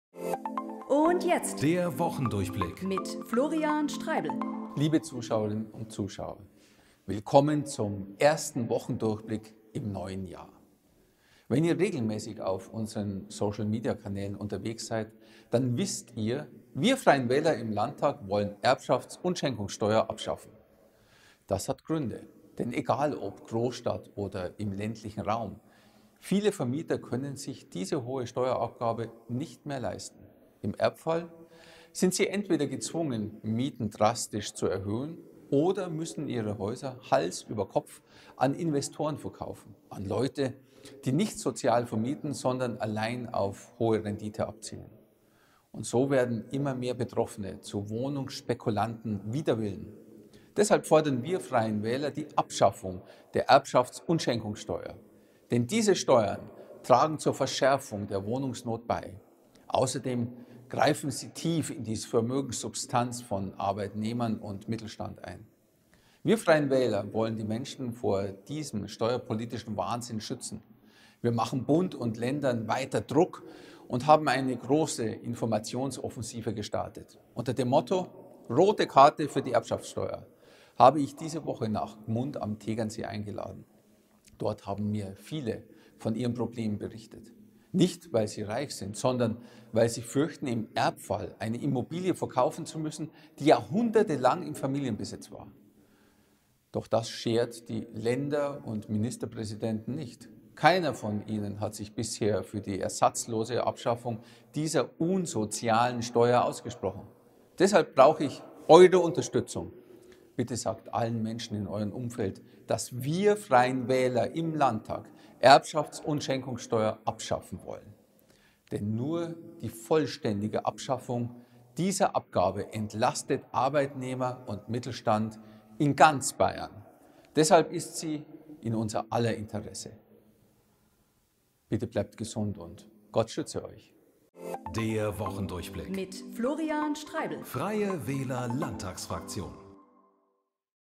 Der Wochendurchblick: Warum wir an unserer Forderung nach Abschaffung der Erbschaftsteuer festhalten
Wir wollen Erbschaft- und Schenkungsteuer abschaffen: Wieso wir uns dafür einsetzen, wen wir damit schützen wollen und vor allem: Wieso wir diese Steuern für von Grund auf unsozial halten, sagt euch Florian Streibl.